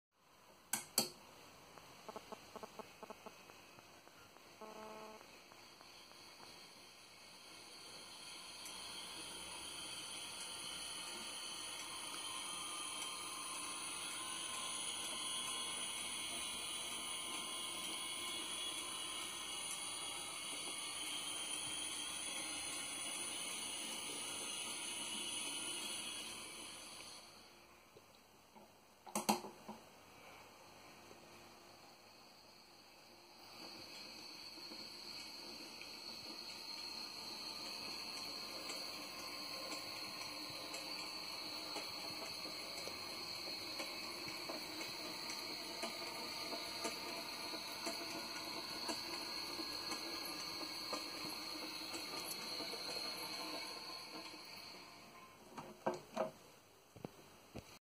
13 September 2020
Early test running of loco built for someone else - hence OO rather than my usual finescale stuff.  Still a little noisy, but some running in and a bit of sound deadening should help.